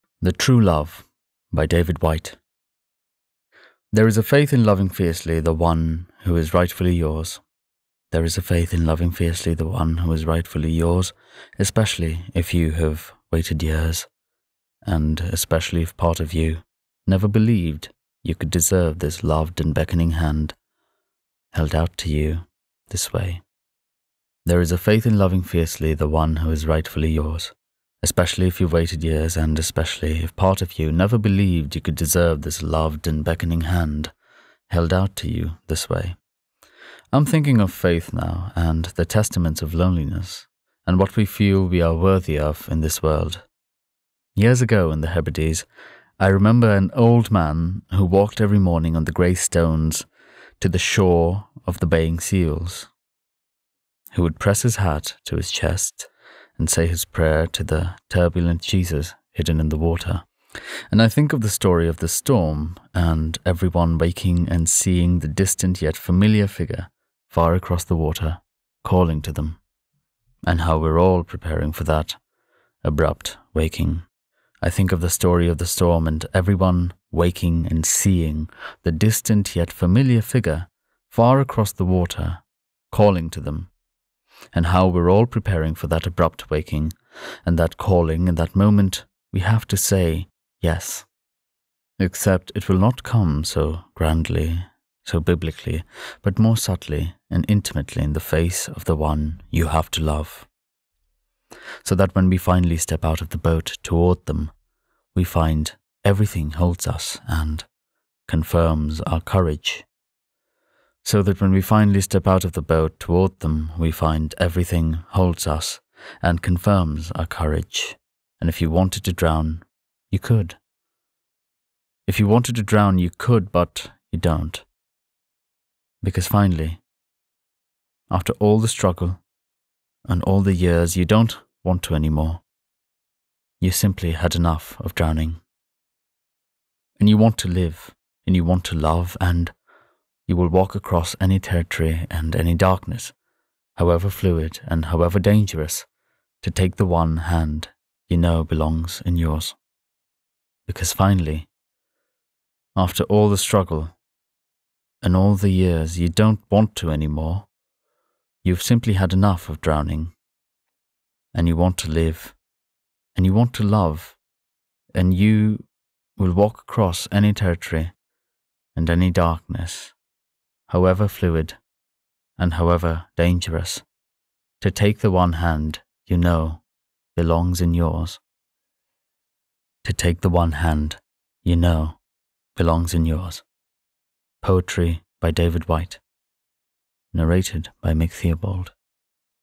The Truelove by David Whyte (read by Mick Theobald)
Poetry by David Whyte. I own no rights.

He reads it way better than I do. But this moves me deeply, and I wished to perform it, if only for myself.